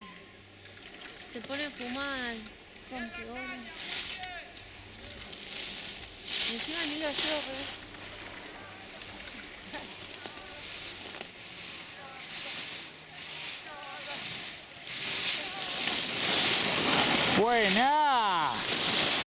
juana y cata
Juana y Cata del grupo de Fonta haciendo un poco de trabajo en equipo!